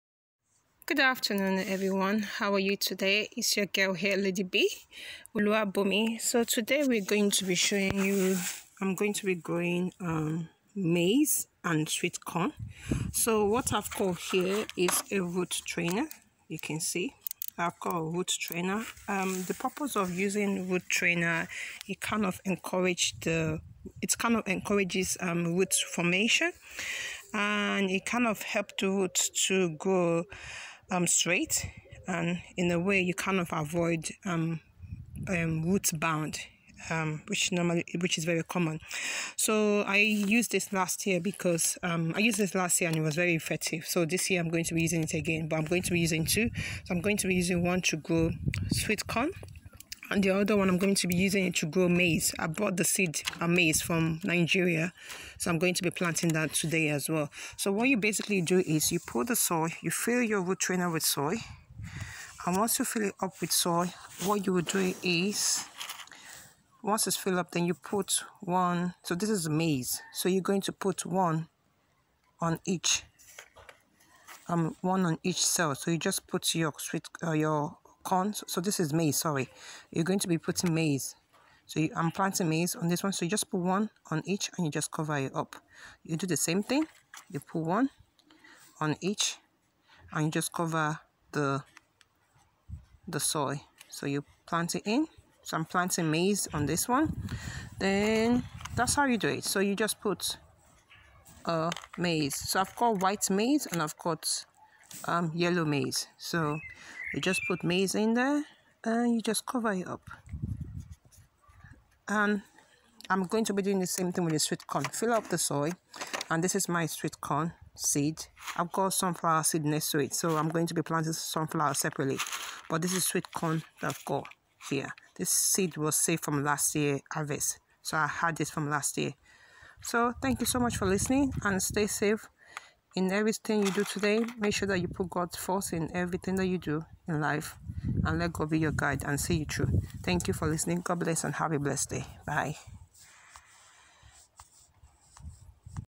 Growing sweetcorn and maize from seed (Part 1) 14.04.20
How I start my sweetcorn and maize seed growing in April using root trainer.  Root trainer encourages a strong and healthy root formation without the risk of root ball.

If you don't have root trainer use the containers that you have. In fact you can use empty kitchen or toilet roll cardboard... Just enjoy nature and Happy Gardening..